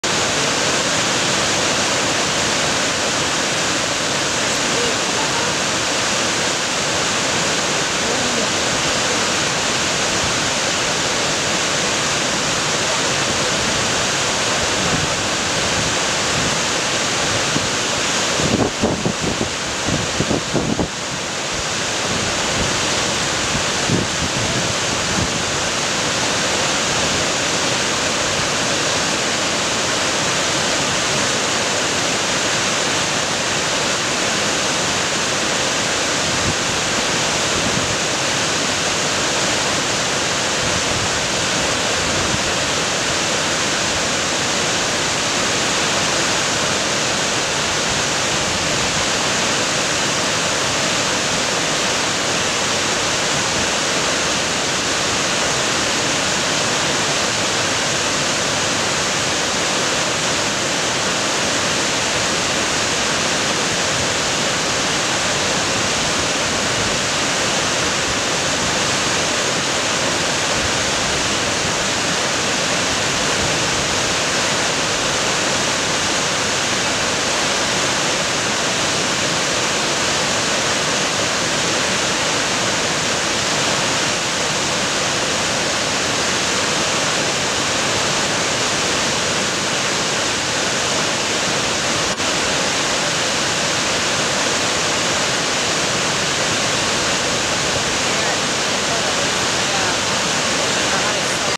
Brown Bears Russian Falls in ALASKA
러시안 폭포에서 브라운 베어 세마리가 연어를 잡는 장면